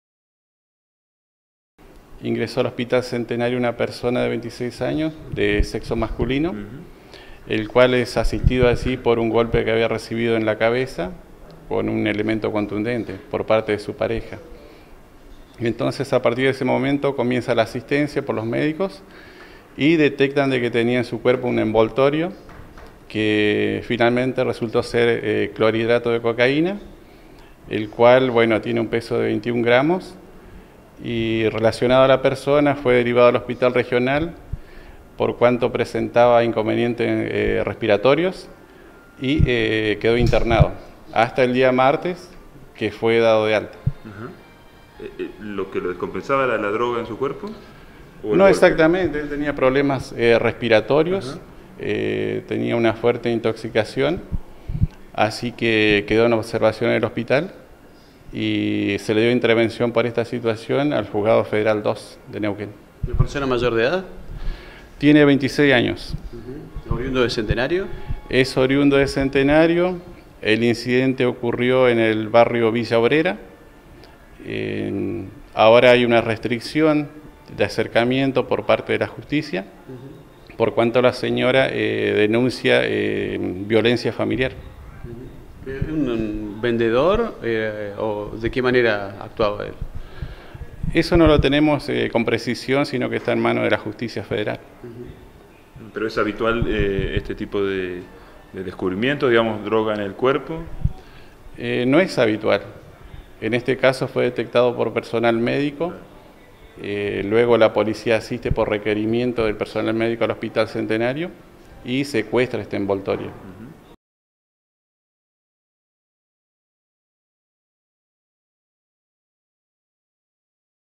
CENTENARIO: PAREJA DE DEALERS A LOS SARTENAZOS
Un joven de 26 años fue hospitalizado con un fuerte golpe en el cráneo, luego de que su mujer le diera un “sartenazo” por quedarse con parte de la droga que iba a ser vendida en un barrio de Centenario. El hombre terminó internado y, además, le encontraron cocaína en el interior de su cuerpo.